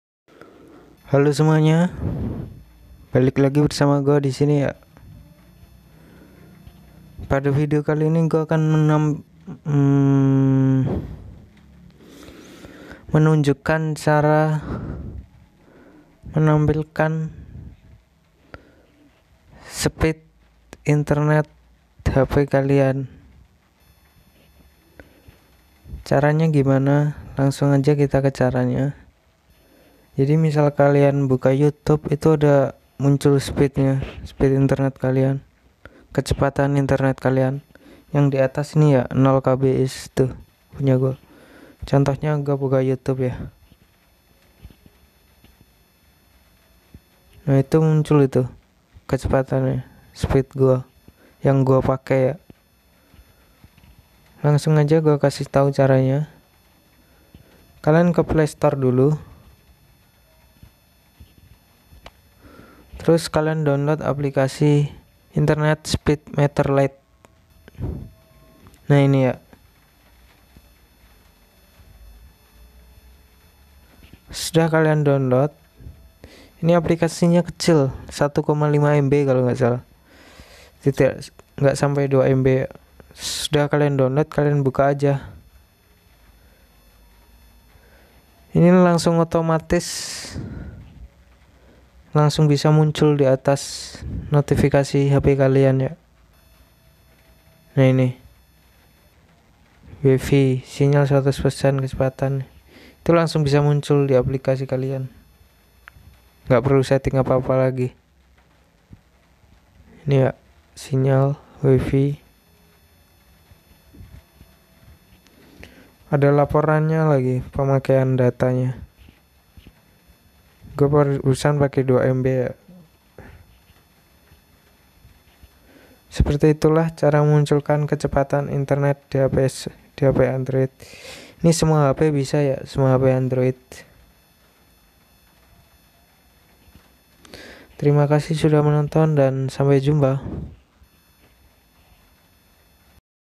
Cara Menampilkan Speed Data Internet di Bar Hp Android
Semua hp cara memunculkan data internet di status bar HP Android. Krownet
krownet review